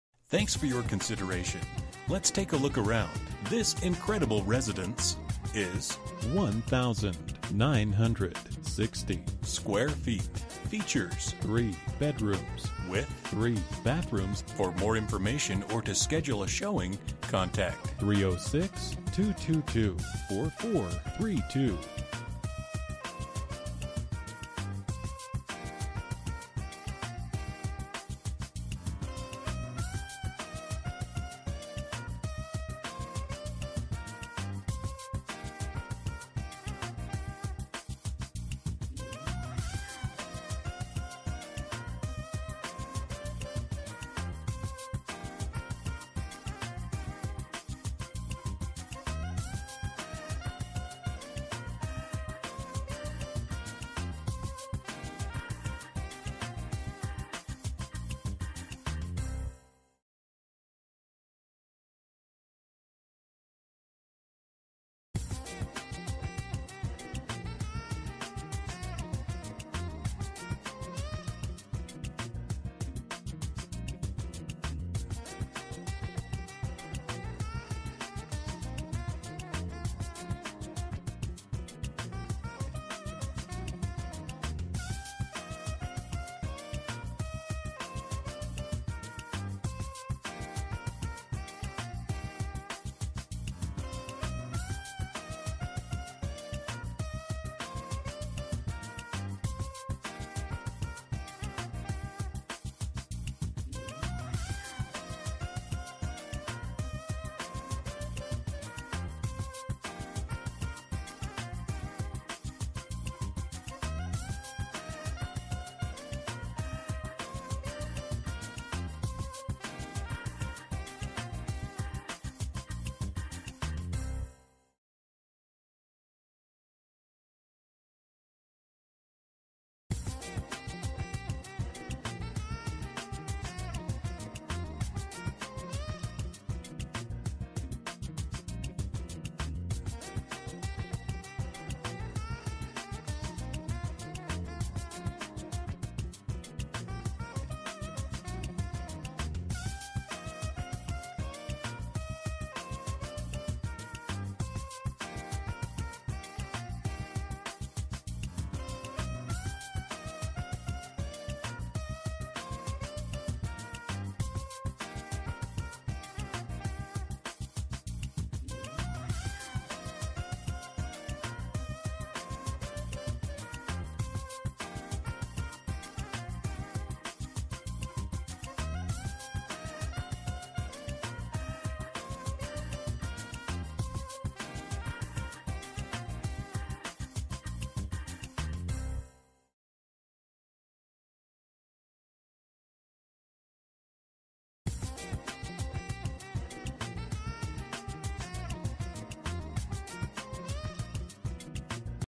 Residential for sale - Rural Address, Days Beach, SK S0M 0L0
Here it is, your chance to own some of the most sought after private-waterfrontage in Saskatchewan. Welcome to #80 Day's Beach, Jackfish Lake, SK. A mere 20km north of North Battleford, this is a once in a lifetime opportunity. Contracted and finished by 'WindRiver Contracting' this home boasts over 2550 square feet of developed space. Features unheard of in this price point are a professional grade chef's kitchen valued at over $140,000, professionally planned and executed HVAC system with in-floor heat, a professionally landscaped yard, 3 entertainment-sized decks with natural gas connectivity in multiple locations, and a view that is absolutely jaw-dropping from the moment you walk in the door. This is an absolute MUST-SEE in person and the fine details of this property will leave you floored. Call us, or call your REALTOR for a private viewing of this unbelievable property.

Bedrooms: 3
Bathrooms: 3
Square Feet: 1960
Price: $1,138,000

MLS ID: SK771277

For more information about this property, please contact Jonathan Katsiris at or . You can also text 5597728 to 67299.

06/09/2019 03:59:28 am

Last modified: